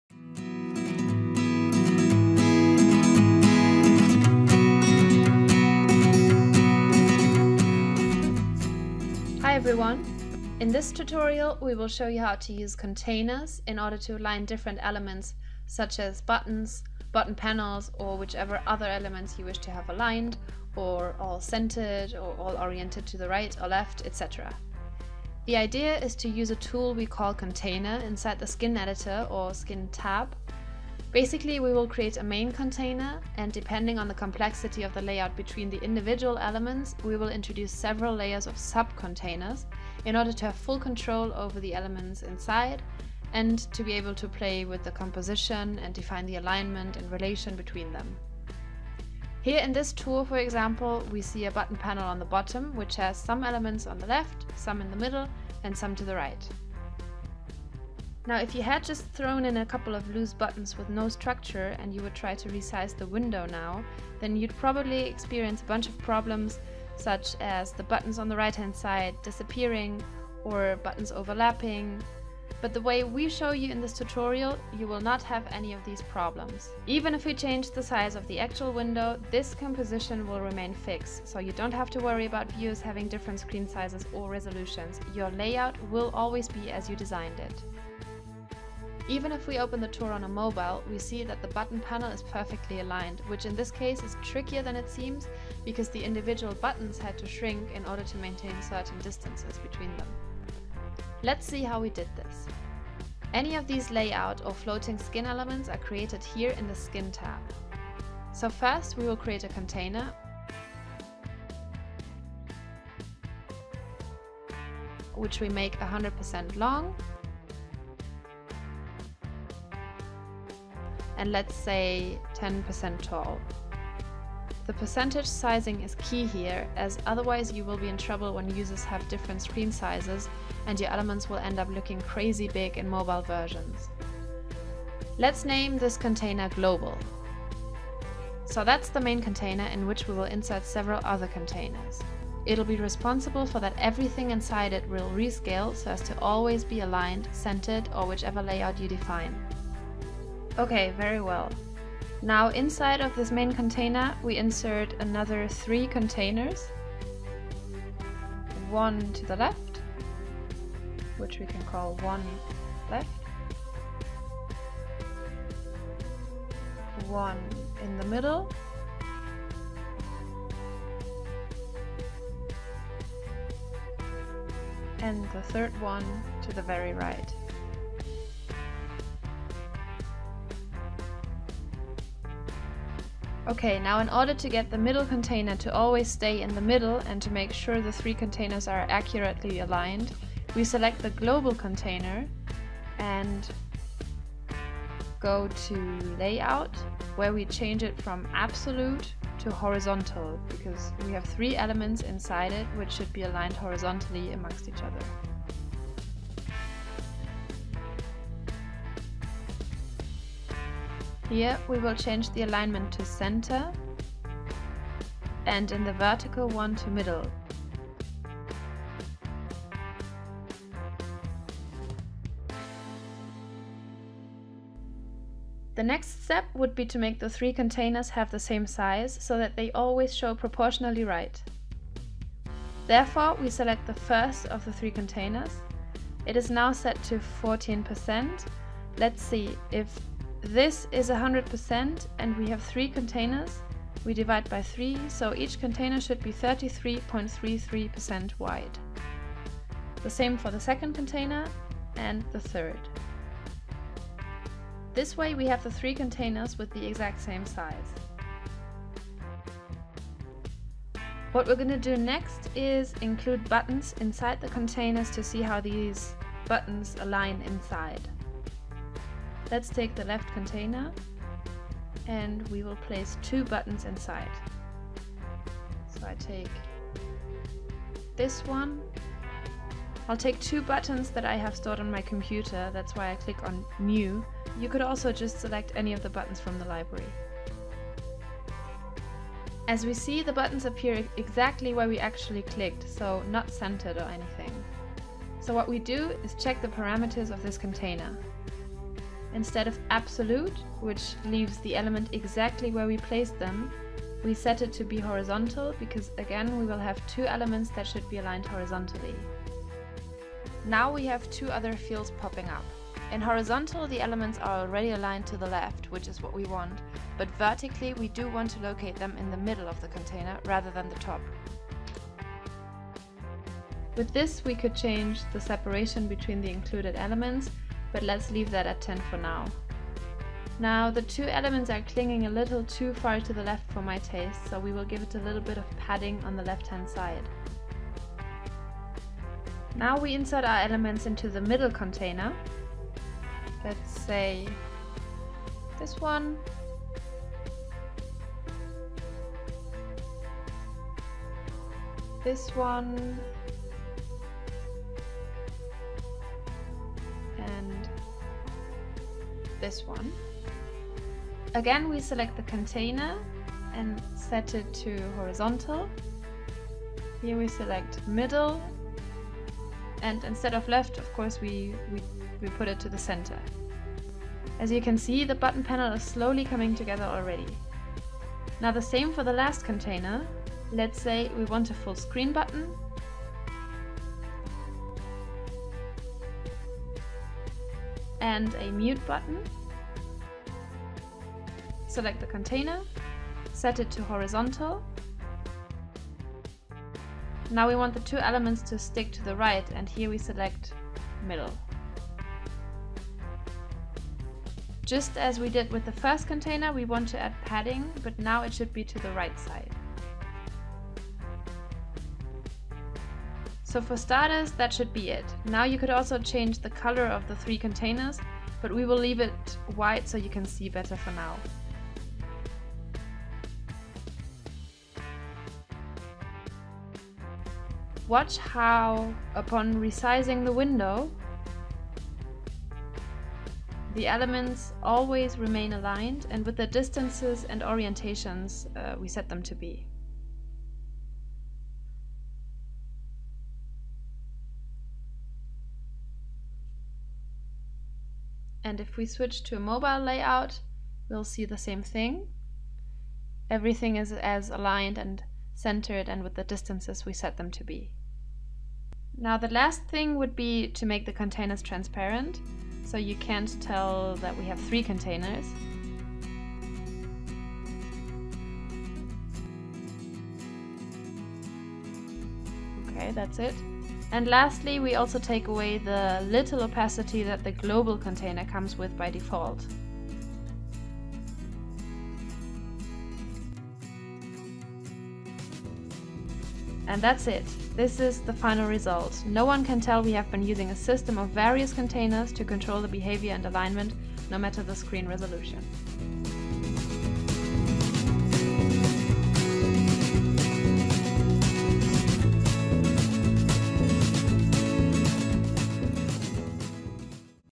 Tutorial: Aligning Skin Elements and Making them Adaptive
This clip shows how to create skin elements that adapt to the screen size that the tour is being viewed on. This way, one and the same skin design can be perfectly viewed on desktop and mobile phones. Using containers, you prevent elements from overlapping or disappearing and your composition remains fix even when resizing the window.